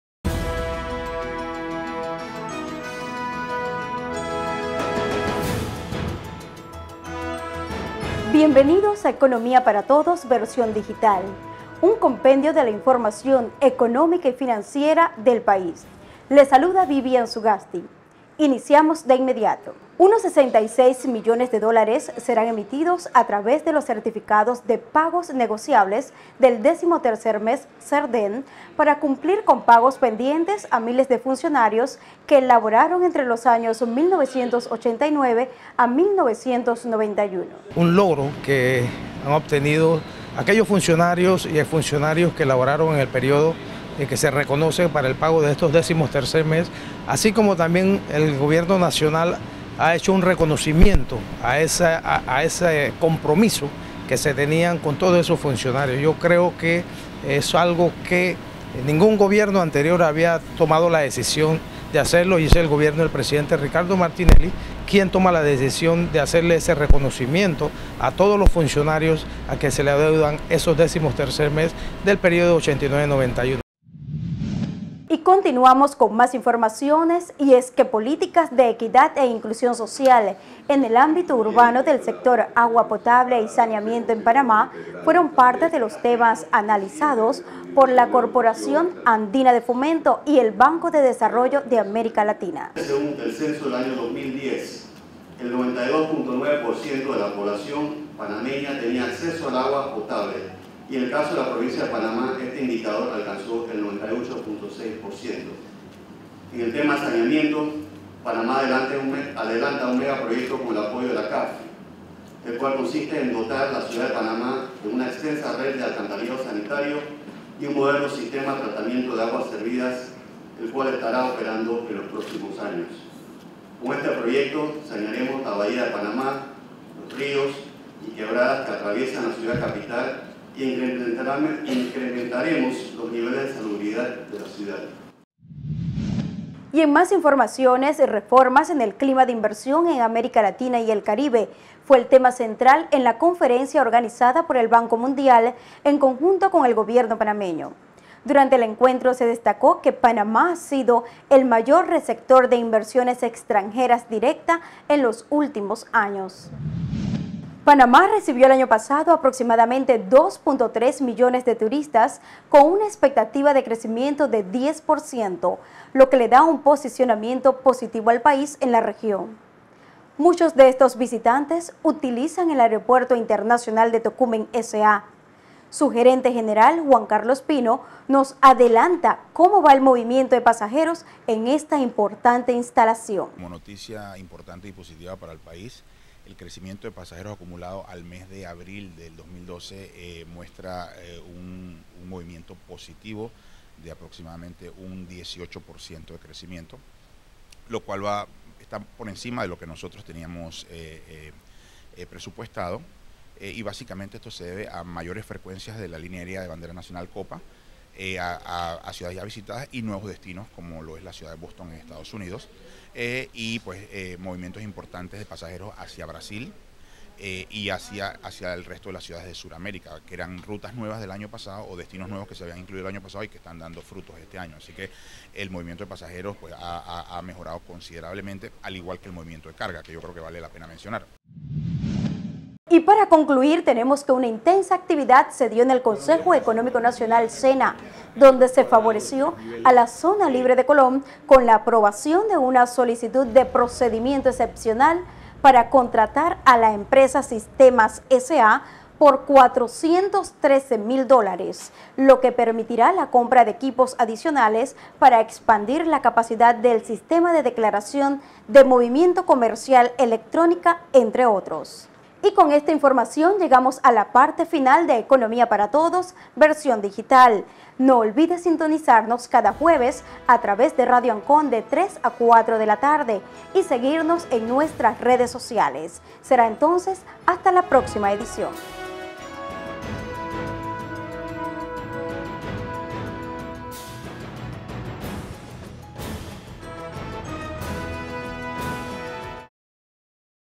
MEF Panamá EPT 7 junio 2013
Noticiero digital del Ministerio de Economía y Finanzas de Panamá "Economía Para Todos". Damos a conocer los planes, programas y diferentes actividades que involucran a la entidad económica y financiera del Gobierno de Panamá.